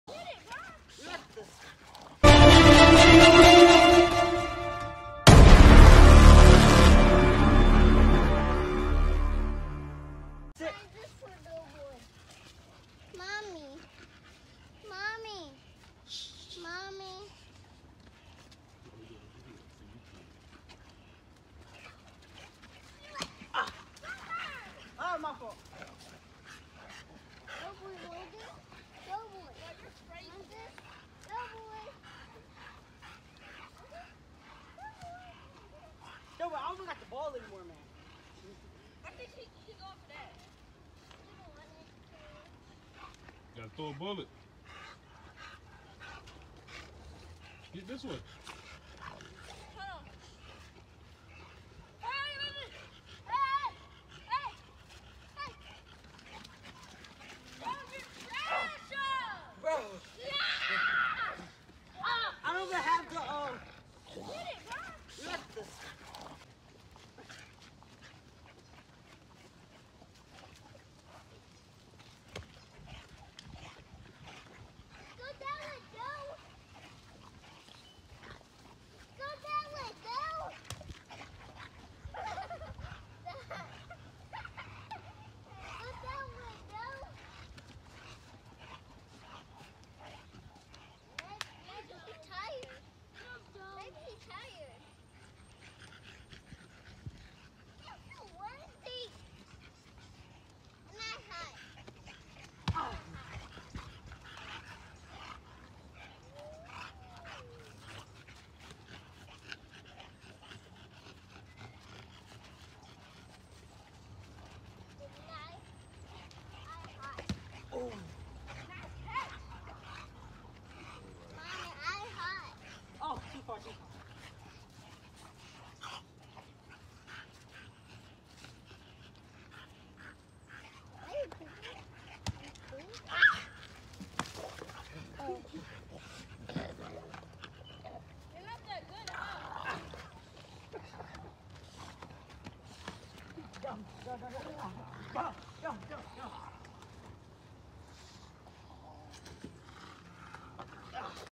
Chocolate Tri Exotic Bully Plays Football with kids
Dough boy is so toy driven, watch as the kids play monkey in the middle with this amazing 16 month old exotic bully. You can also see French Bulldog Butter trying to get in on the action.